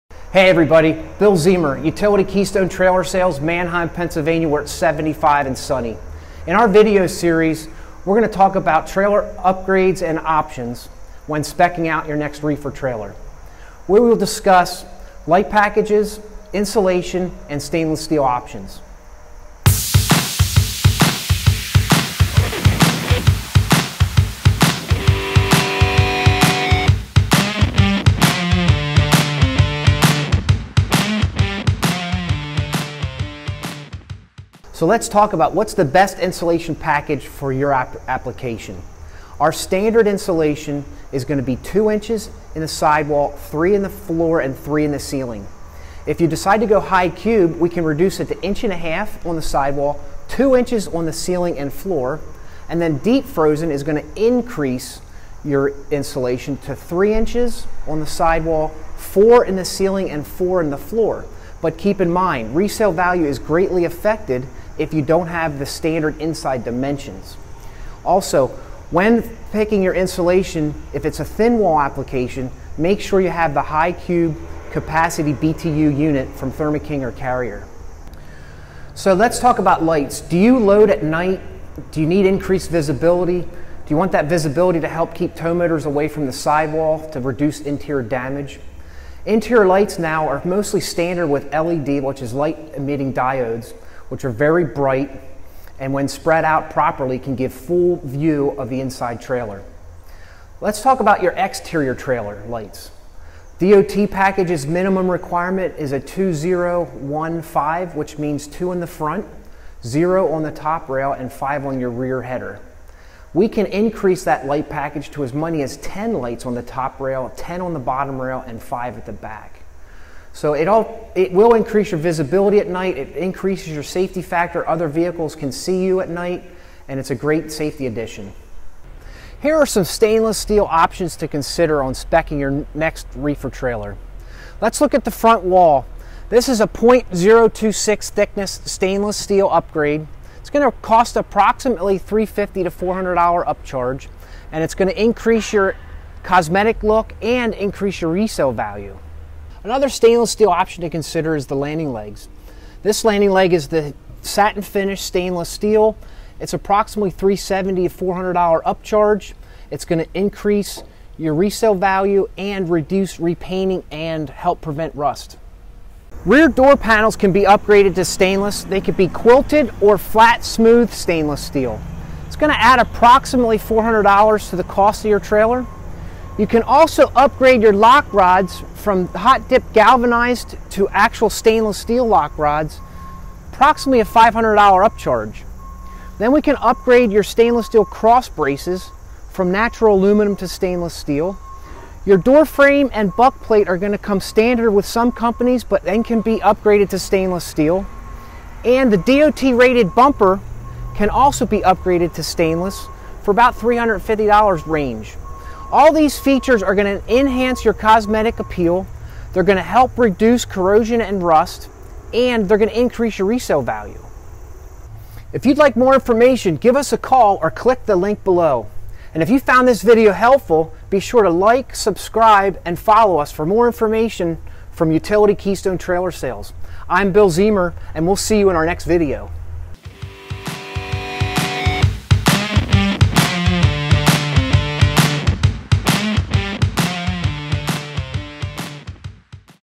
Insulation, Lights, & Stainless Steel Upgrades & Options | How to Buy the Best Reefer Trailer
In this video, we’ll discuss trailer body features and upgrades you’ll want to consider when spec’ing your next reefer trailer.

Today we’re talking about insulation, light packages, and stainless steel options.

What Thickness Insulation Do I Need For My Reefer Trailer?
More insulation means greater ability to hold temperatures, and more insulation means thicker walls, which reduces interior space.

Common insulation thicknesses:
2” on the side  |  3” on the roof  |  3” in the floor  |  4” on the front wall  |  3" in rear doors

Thin Insulation: 1.5” on the side  |  2” on the roof  |  2” in the floor  |  4” on the front wall
Building your reefer trailer with Thin Insulation in walls, and ceiling, and floor you can increase the interior space.

Light Package Options For Reefer Trailers.
• Do you load or unload at night?
• Do you want ceiling lights?

Interior Lights:
• If you’re loading at night, you’ll need interior lighting to keep drivers and dock workers safe and make reading easier.
• Trouble-free and long-lasting Light-Emitting Diodes (LEDs) produce a white light that spreads throughout the interior of a trailer.
• LEDs are installed in 90 percent of new refrigerated trailers.

Exterior Lights:
• Minimum DOT requirements: is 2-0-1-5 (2 on Front, 0 on Top Rail, 1 on Bottom Rail, 5 on Rear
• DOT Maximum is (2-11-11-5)

Stainless Steel Features and Options for Semi-Trailers
Stainless Steel helps prohibit corrosion. Upgrade these components:
• Front Panel
• Rear Doors
• Wing Plates
• Rear Bumper

Questions? Comments? Ideas? Leave them in the comment section below!